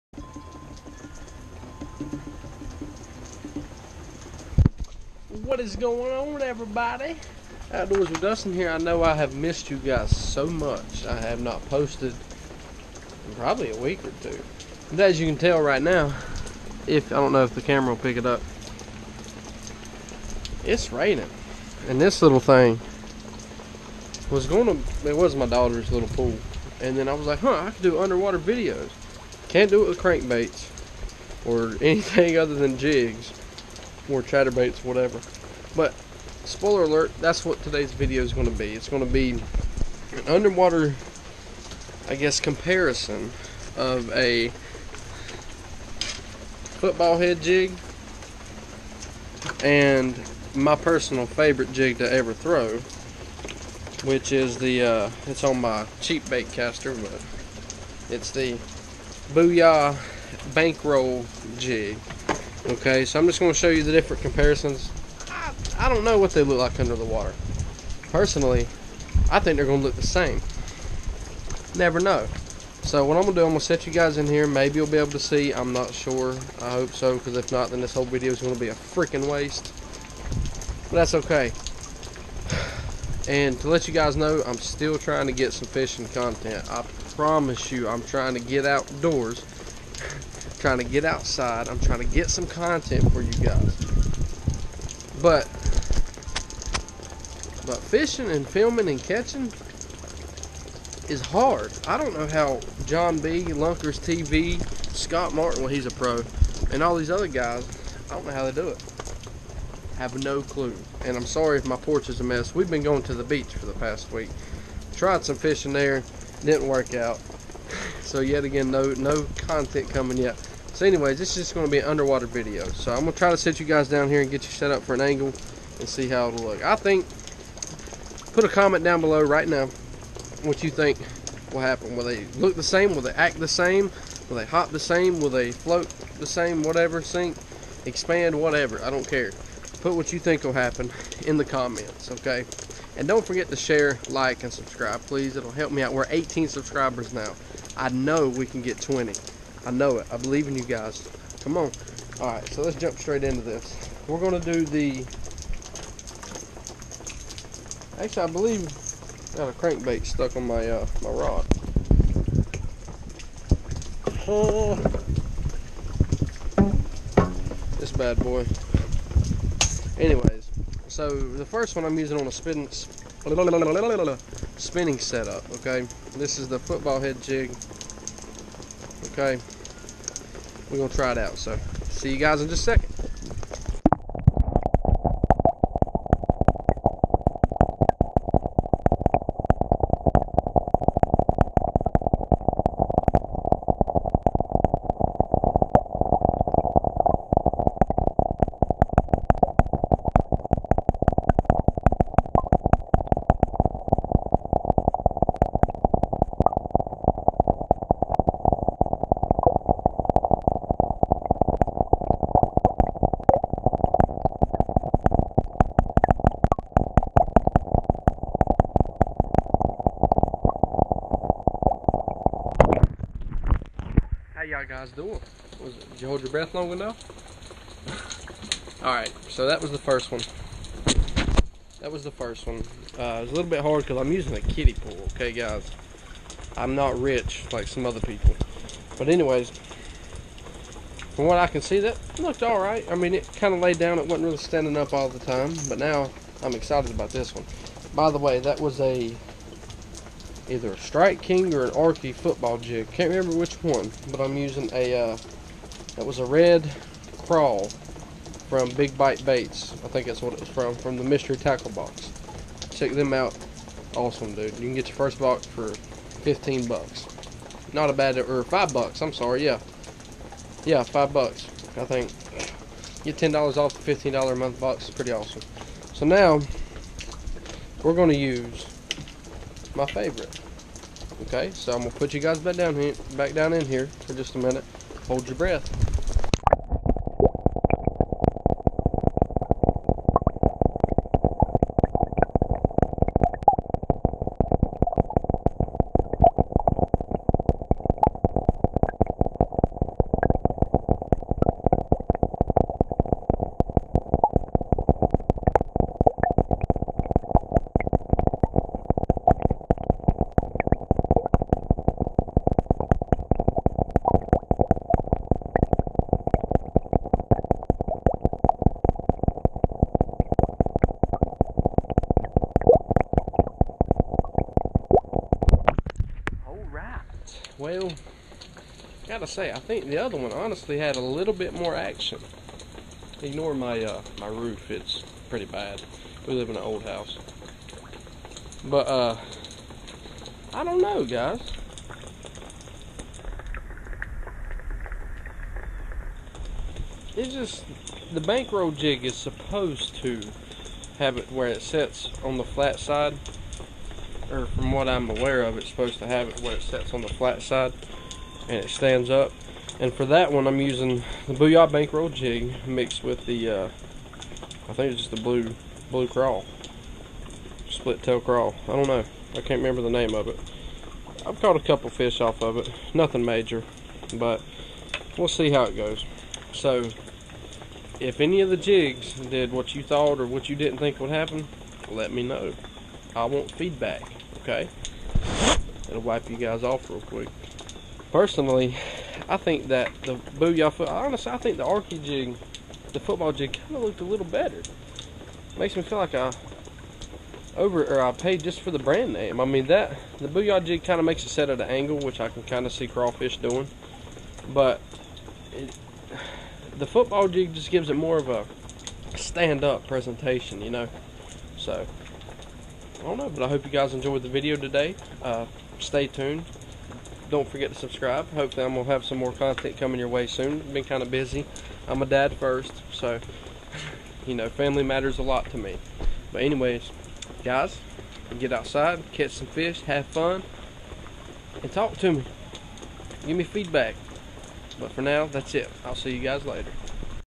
Underwater lure battle! Booyah vs. Strike King!
Underwater video comparison of booyah bank roll jig and the strike King football head jig, also I apologize for the lack of content. Fish don't seem to bite when I film! Also, ignore the mess. I was gone with family for a week to the beach and a couple severe storms rolled through the area.

HOLY COW GUYS! That's awesome, now let's hit 20! I know we can do it! And let's get this video to 5 likes!